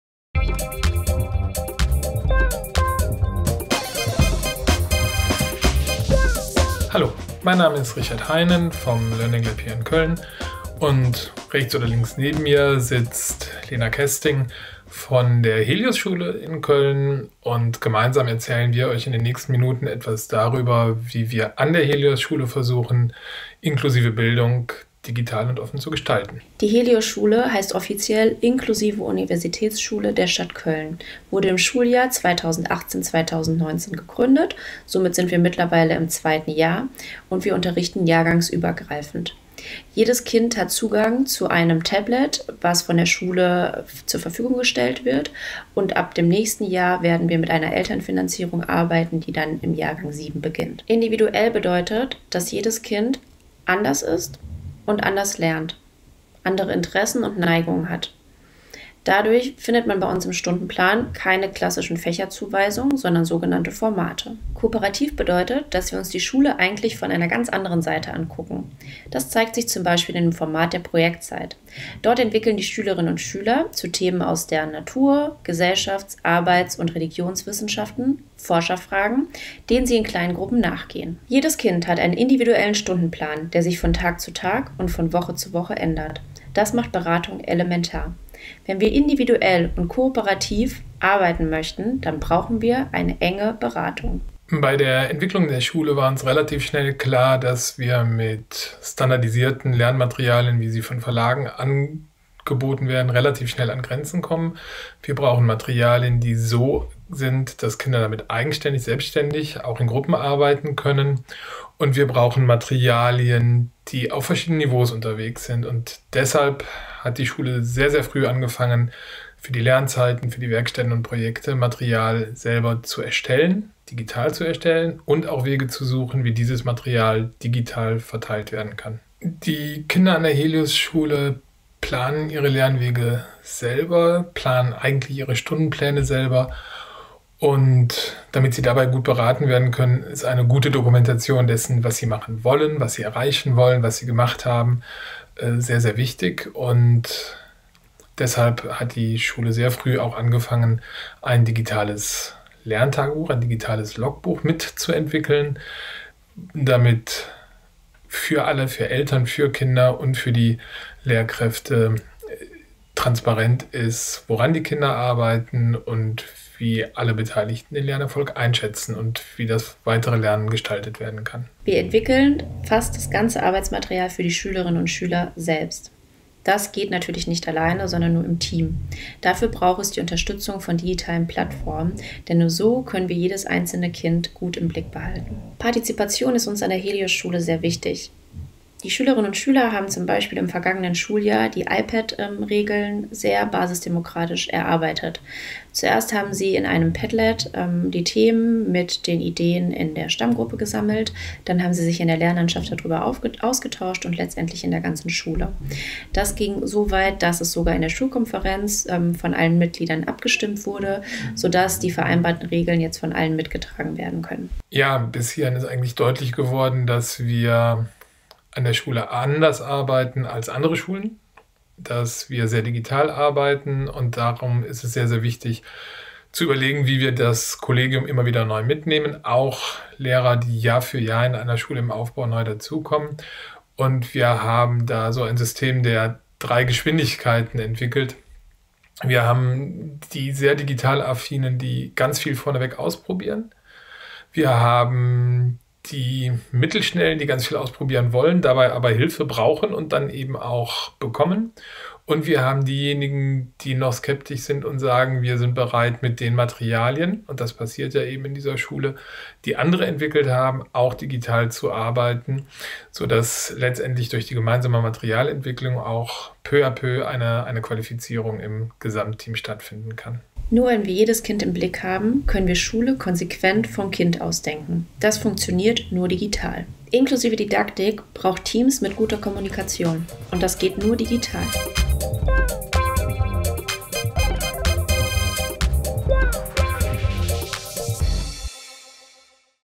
Inklusive Didaktik - digital & offen | Edunautika-Lightning Talk von Richard Heinen und Lena Kesting
Warum und wie inklusive Bildung und Didaktik nur offen digital gelingen können, ist das Thema im Lightning Talk von Lena Kesting (Lehrerin, Heliosschule Köln) und Richard Heinen (Schulentwickler, learninglab). In ihrem Talk erzählen sie davon, wie das an der Heliosschule Köln umgesetzt wird.